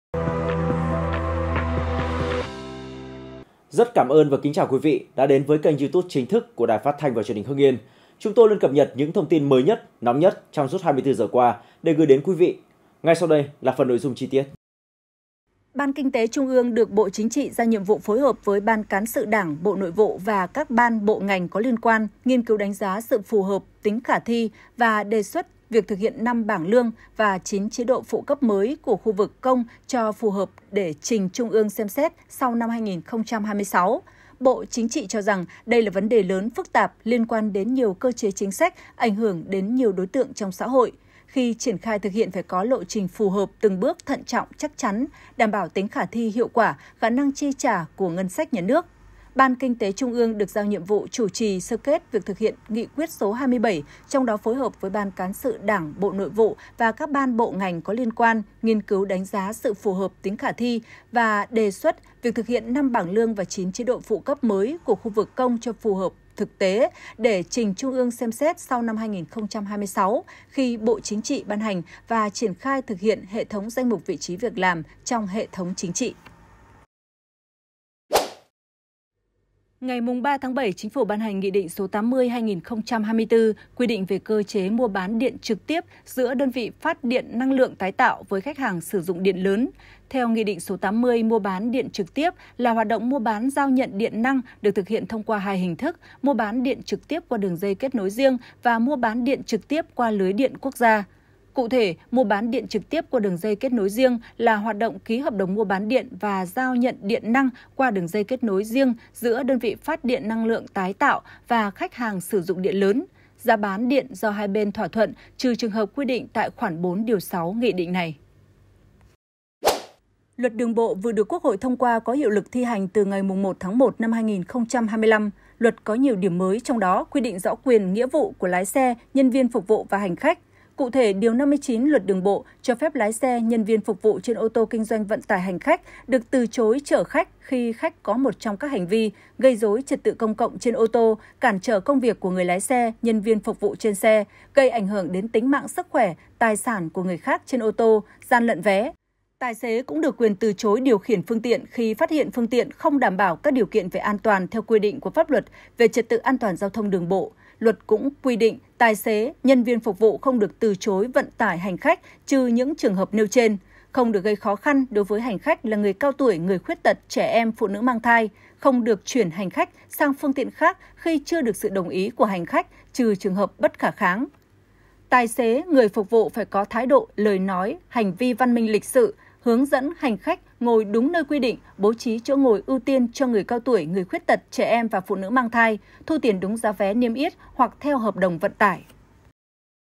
Đề xuất 5 bảng lương, 9 chế độ phụ cấp mới để trình Trung ương xem xét sau năm 2026
Hưng Yên TV | Đề xuất 5 bảng lương, 9 chế độ phụ cấp mới để trình Trung ương xem xét sau năm 2026
Địa chỉ: 164 Nguyễn Văn Linh, thành phố Hưng Yên, tỉnh Hưng Yên
Tiktok: Truyền hình Hưng Yên
Facebook: Đài Phát thanh và Truyền hình Hưng Yên
Zalo: Đài PTTH Hưng Yên